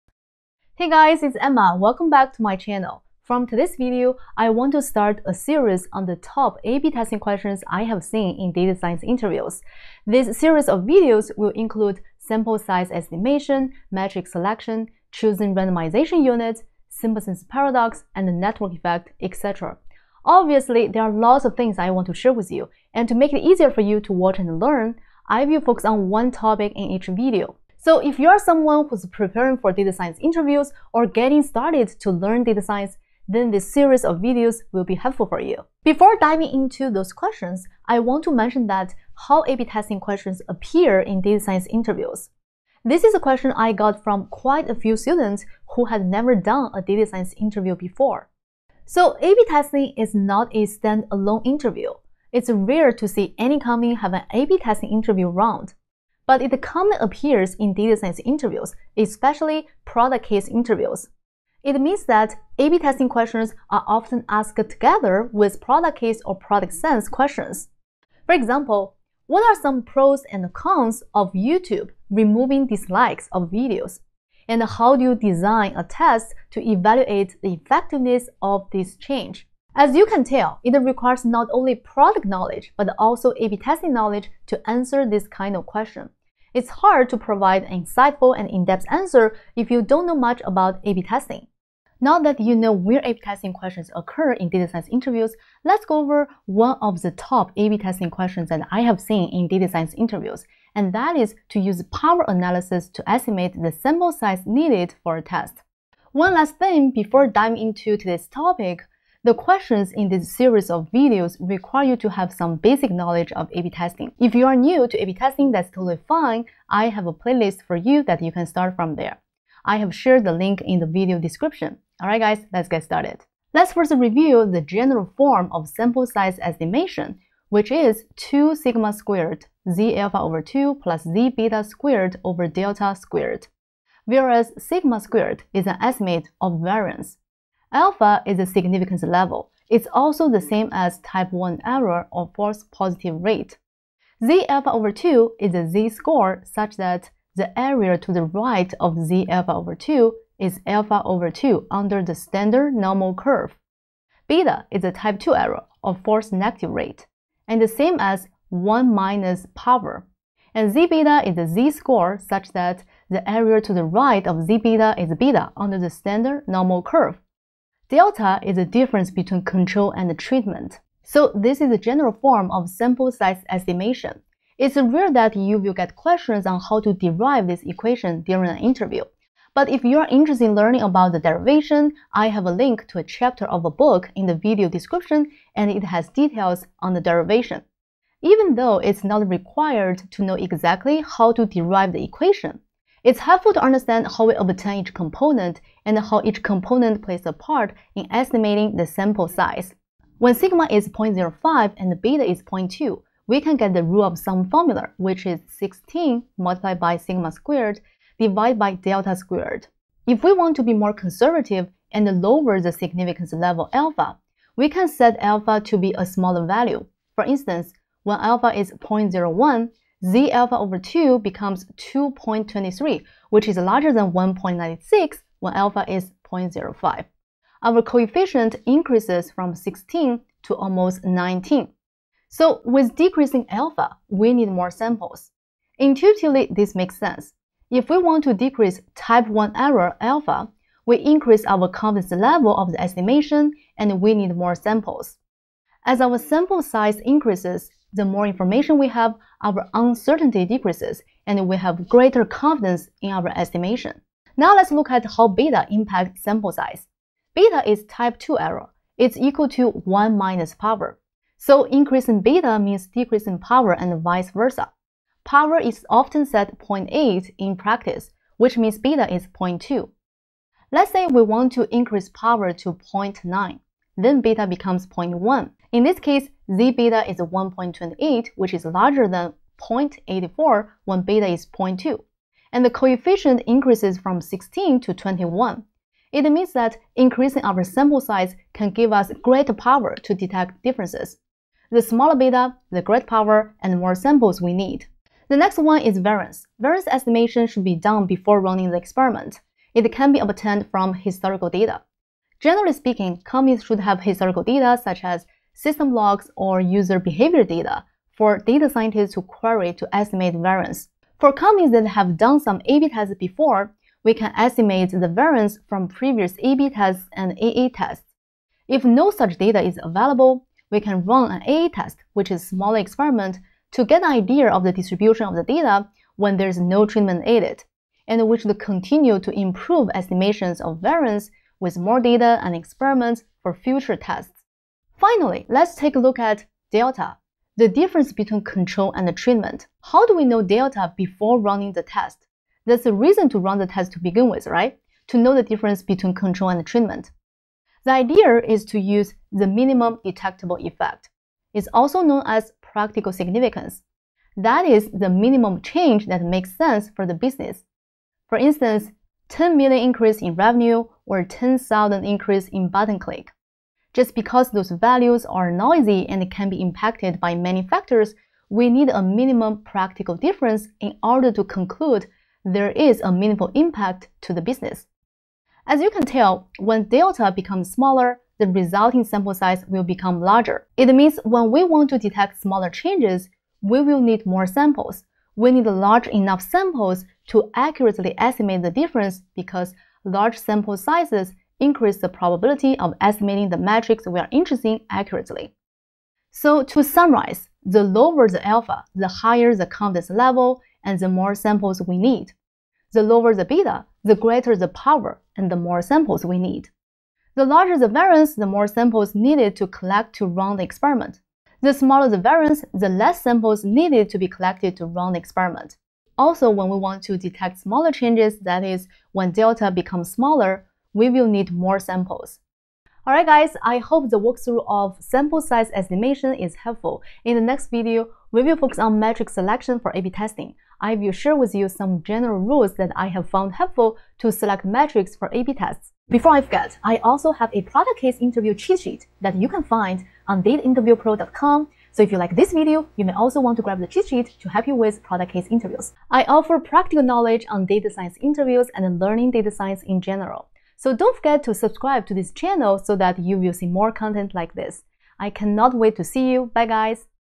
Sample Size Estimation in A/B Testing: Easy Explanation for Data Science Interviews
In this video, we will go over one of the top A/B testing questions I've seen in data science interviews: how to estimate sample size in A/B testing? This is the first video of a video series - Top A/B testing questions in Data Science interviews.

✔️ Derivation of sample size equation - Statistical Rules of Thumb - Chapter 2: Sample Size

✅ We work with Experienced Data Scientists to help them land their next dream jobs. Apply now

 Contents of this video:
0:00 Intro
0:40 How A/B Testing Questions Appear in Interviews
2:03 Rule of thumb formula
3:20 Alpha (Significance Level)
4:15 Beta (Type II Error)
4:56 Variance
5:36 Delta
6:36 Summary